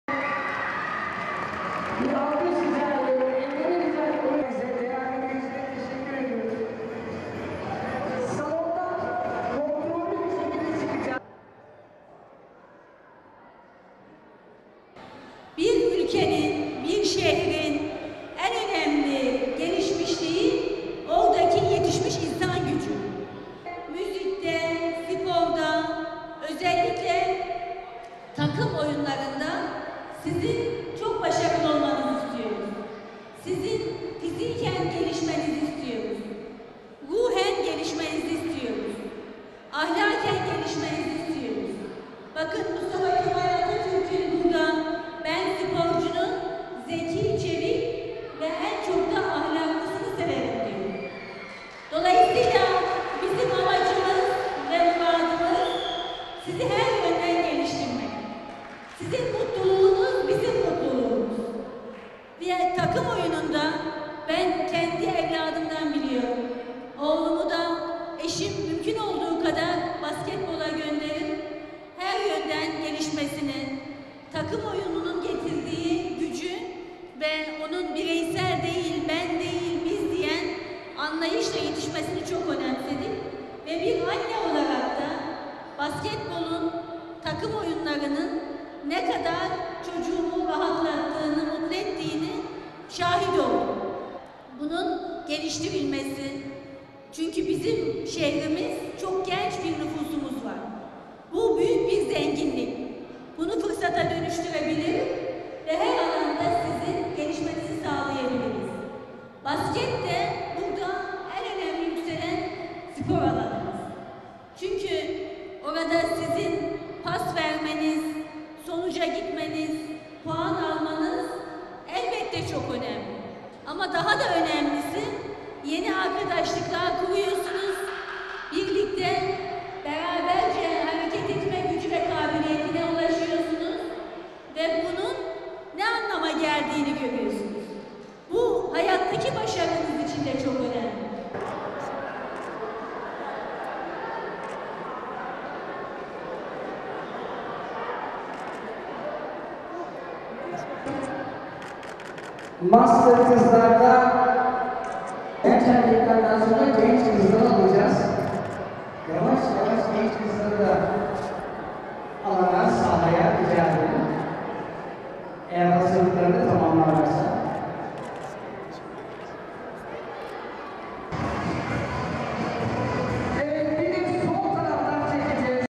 SOKAK BASKETBOLU ÖDÜL TÖRENİ
11. Sokak Basketbolunda ödüller sahiplerini buldu.
Spora ve sporcuya destek vermeye gençleri spor konusunda bilinçlendirmeye devam ediyoruz.